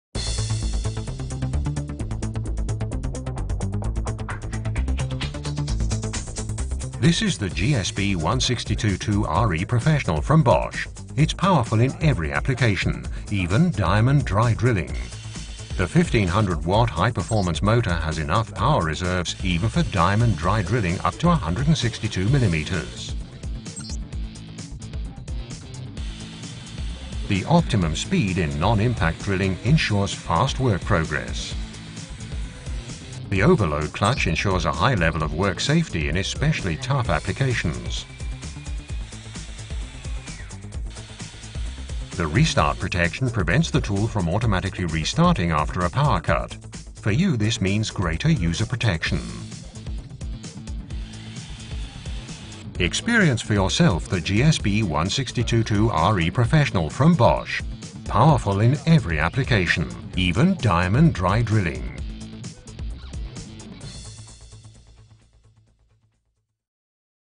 Bosch Impact Drill | GSB 162-2 RE Professional
Bosch GSB 162-2 RE Professional Impact Drill provides the powerhouse in any application even when drilling dry diamond! This Bosch impact drill is a very powerful drill even masonry core drilling up to 162 mm and it can be used for vast number of application as it is highly versatile for applications in wood, masonry and metal due to impact mechanism and 2-speed gearbox with wide range of rpm. It has high durability Metal gear housing and 7 C Motor with ideal heat management. Its high torque makes it easy to work on power-demanding applications. It includes a high-performance motor (1500W) with high overload capacity for the toughest applications. It consists of an overload clutch with torque level of approx. 50 Nm. It includes a restart protection mechanism which prevents the tool from automatically restarting after a power cut. It includes a robust auxiliary handle with innovative fixing and soft grip for a secure hold on the machine. Its robust 16mm keyed chuck with automatic locking. It consists of a speed preselection mechanism with setting wheel which provides precision while working. Technical Data: Rated power input 1.500 W No-load speed 0 – 750 / 2.550 rpm Power output 840 W Weight 4,8 kg Rated speed 0 – 555 / 1.890 rpm Torque (soft screw driving applications) 50,0 / 14,5 Nm Rated torque 17,0 / 5,0 Nm Drill spindle connecting thread 5/8" – 16 UN Chuck capacity 3 – 16 mm Length 452,0 mm Height 176,0 mm Impact rate at no-load speed 0 – 12.750 / 43.350 bpm Drilling diameter in concrete 82 / 68 mm Drilling diameter in wood 50 / 30 mm Drilling diameter in steel 20 / 14 mm Drilling diameter in masonry 162 / 68 mm.For more information, please visit our